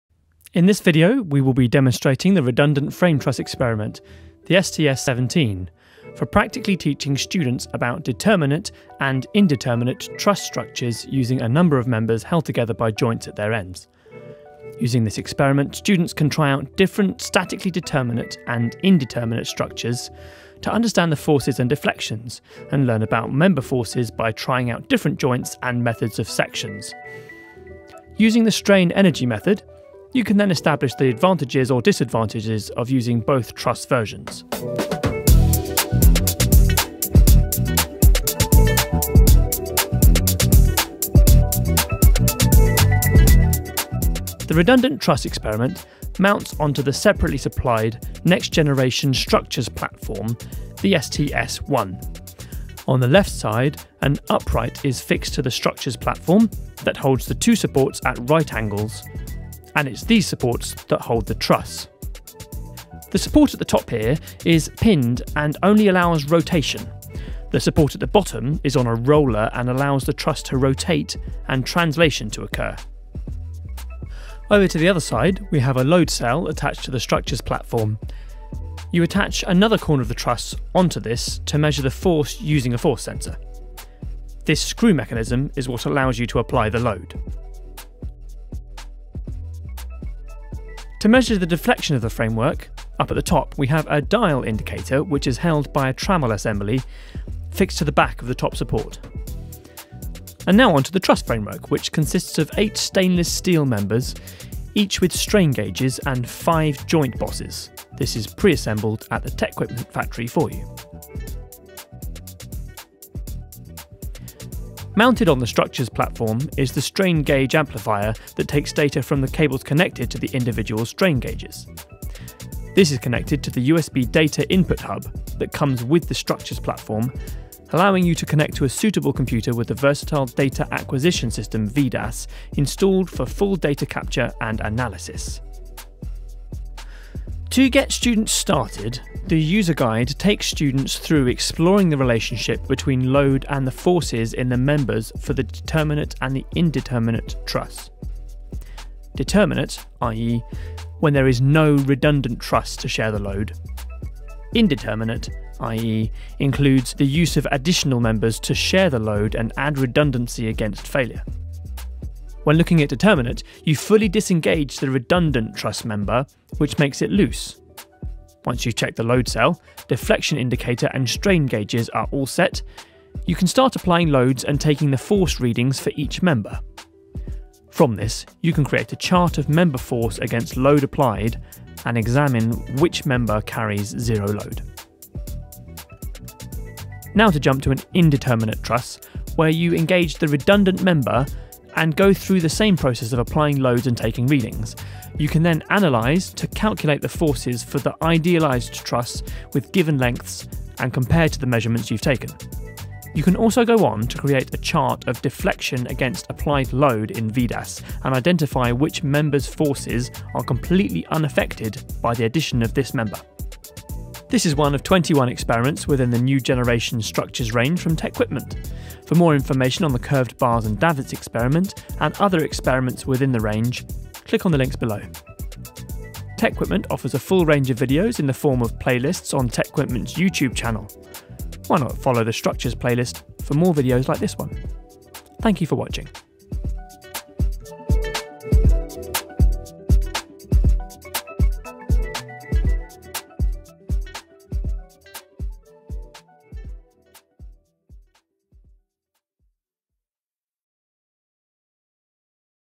Redundant Truss Experiment (STS17) - Next Generation Structures - TecQuipment
In this video we will be demonstrating the Redundant Truss Frameworks Experiment, the STS17, within the Next Generation Structures range of engineering teaching equipment from TecQuipment.

This experiment allows the study of determinate and indeterminate truss structures.

Many structures use frameworks or trusses as they are relatively easy to construct, lightweight and rigid. In civil engineering, you might have come across the Warren Truss and Roof Truss. Space Frames used in vehicles and aircrafts are another example. Naturally, in all of these industries engineers need to understand the external forces and resulting deflections to prevent failure and possible disaster.

More information on the Curved Bars and Davits Experiment (STS14) follow the

00:00 Introduction
00:16 Using this experiment students can learn about forces and deflections and member forces
00:45 The Redundant Truss Experiment set up
01:43 The Truss Framework
01:55 Strain Gauge Amplifier
02:15 User Guide usage
02:29 Determinant Truss
02:34 Indeterminant Truss
03:35 TecQuipment's Next Generation Structures Range
03:55 Thanks for Watching